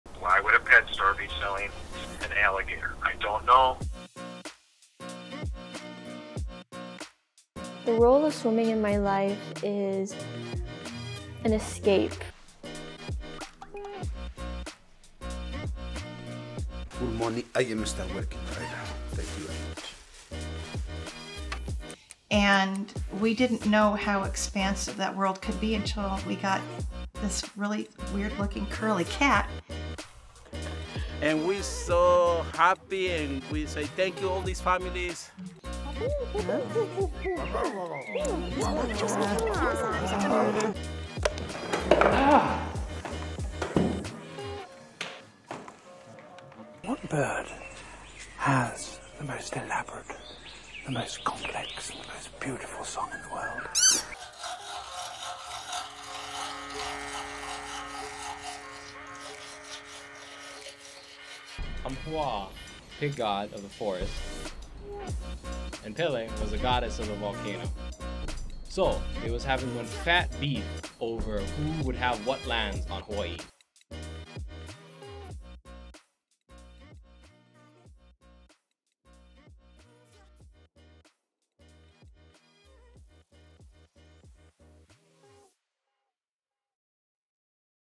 Pacific University Film & Video Showcase
The students of the Pacific University Media Arts department work hard to create amazing and memorable videos and were generous enough to showcase them this week as part of our inauguration celebration! Catch a quick glimpse of their work, and keep your eyes peeled for more content from these star students.

Learn more about the film and video major at Pacific University